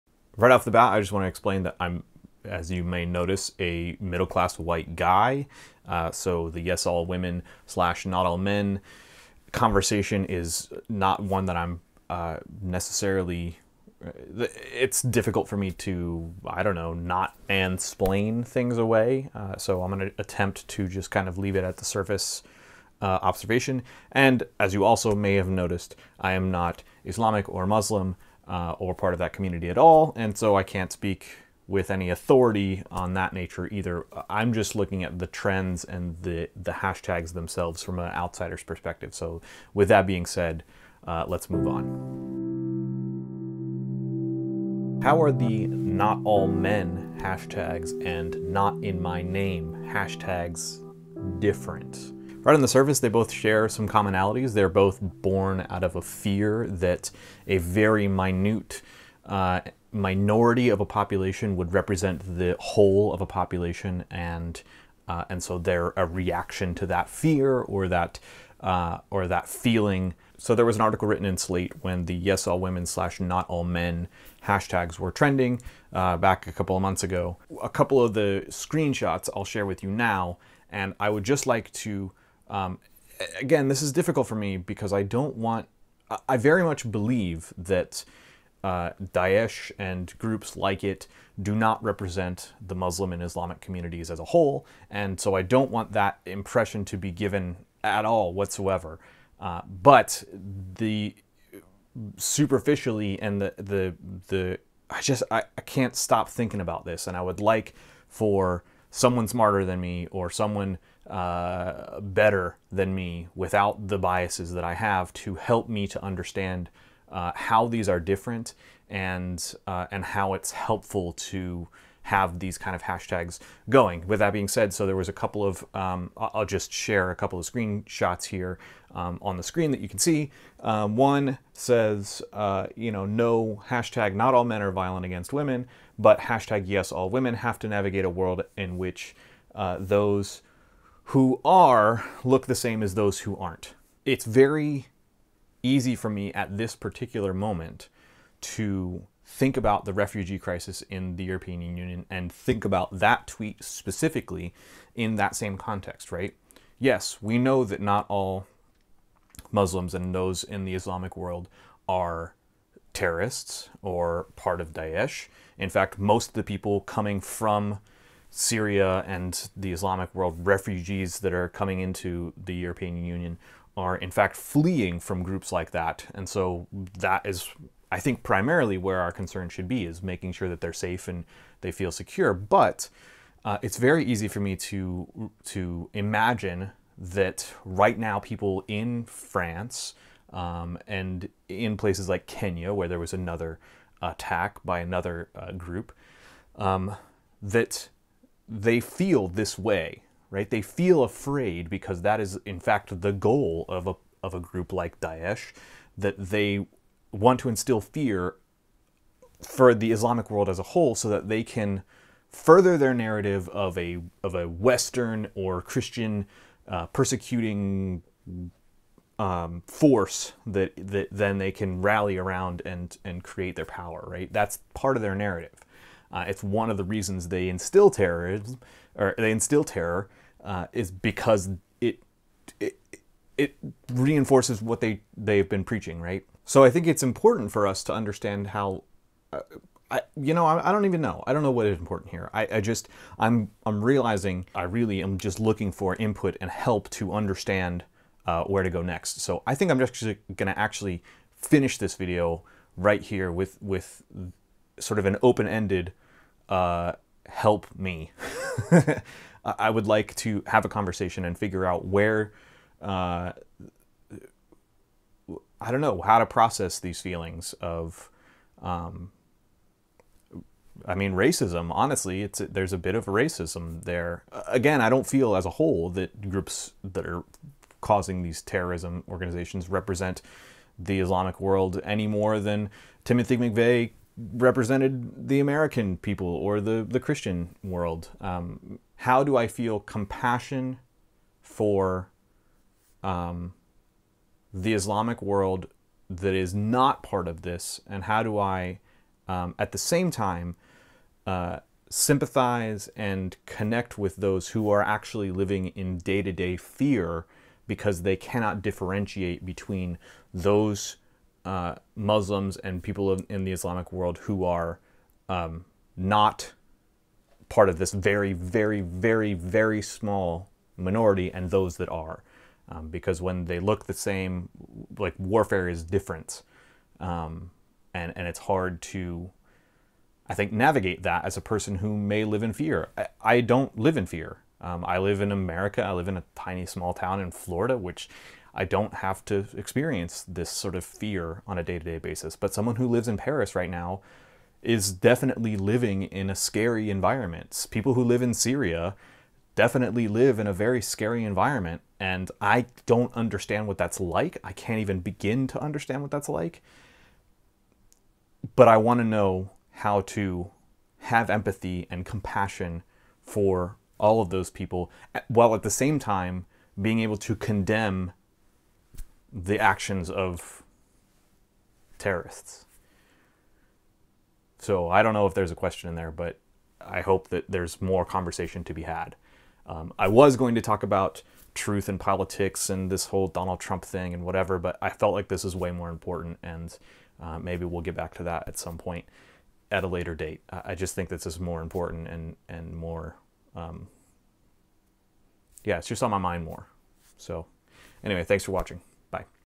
NotInMyName & NotAllMen, Fallout from Attacks in Paris
After the Terrorist Attacks in Paris, Kenya, and Beirut during the November 14-16th 2015 weekend many in the Islamic Community took to Twitter, Instagram, Facebook, and Snapchat to spread word that those attacks were NotInMyName and that groups like Daesh (formerly called ISIS, ISIL, or the "Islamic State") do not represent the faith of the majority of Muslims in the world. When I saw this come across my feed on Facebook, shared from Mashable, I immediately thought about the NotAllMen hashtag that went around awhile ago, and felt terrible that my mind had drawn the connection, so I needed to process a bit.